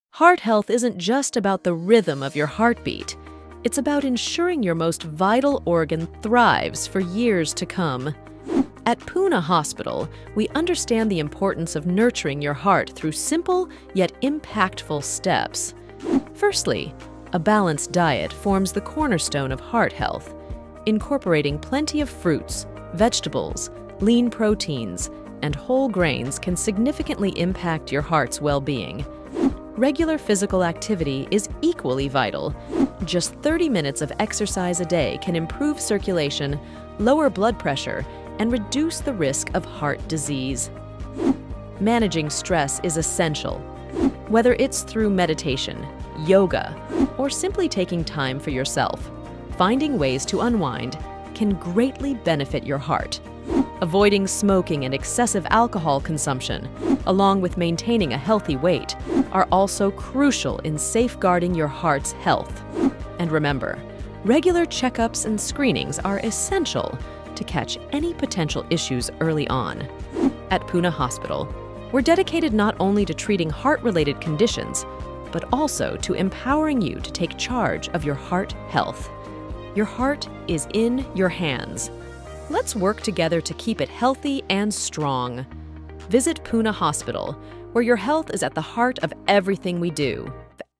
Take charge of your heart health with Poona Hospital. From balanced nutrition to stress management
At Poona Hospital, we prioritize heart health, recognizing it as essential for long-term well-being. We advocate for a holistic approach, emphasizing balanced nutrition, regular exercise, stress management techniques like meditation or yoga, and avoiding harmful habits like smoking and excessive drinking. By incorporating these simple yet impactful steps into your lifestyle, you can proactively care for your heart. Regular check-ups and screenings are also emphasized for early detection of any potential issues. At Poona Hospital, we're committed to empowering individuals to take control of their heart health and ensure a thriving future for their most vital organ.